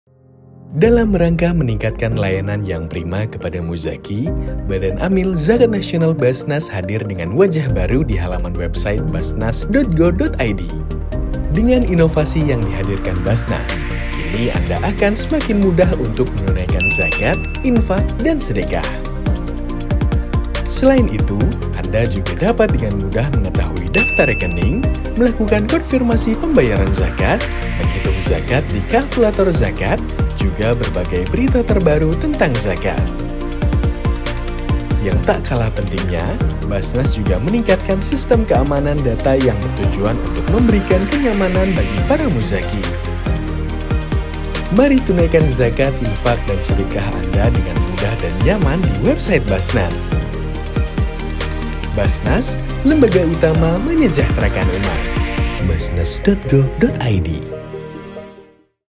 TAMPILAN BARU WEBSITE BAZNAS
sudahkah sahabatBAZANAS menjelajahi pengalaman baru dalam dunia digital kami?

BAZNAS dengan bangga mempersembahkan website terbaru yang telah diperbarui dengan desain yang modern dan fitur canggih untuk meningkatkan layanan BAZNAS kepada muzaki.

Mari bergabung dengan kami dalam petualangan tak terlupakan, di mana Anda akan menemukan: Desain yang Mengagumkan, Fitur Inovatif, Penawaran Spesial, Sistem Keamanan dan Pengalaman Pengguna Luar Biasa.

Jadi, tunggu apalagi? Nikmati kemudahan lebih untuk berbagi kepada mereka yang membutuhkan melalui halaman